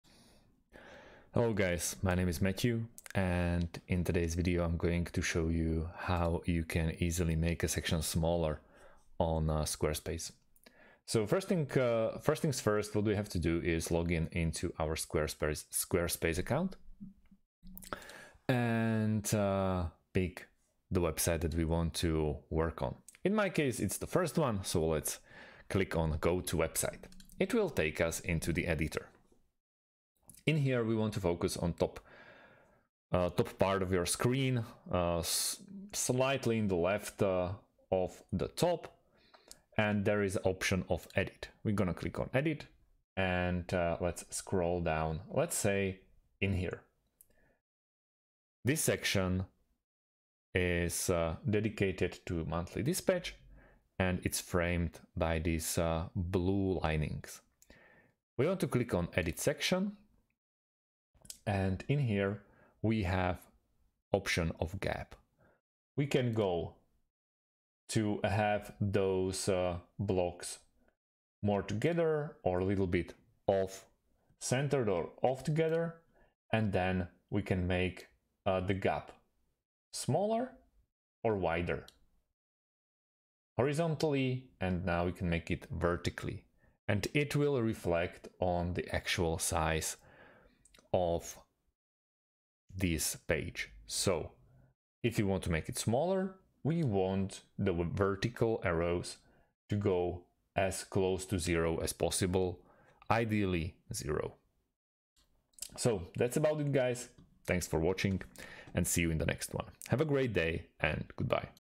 How To Make A Section Smaller On Squarespace
In This Video You Will Learn How To Make A Section Smaller On Squarespace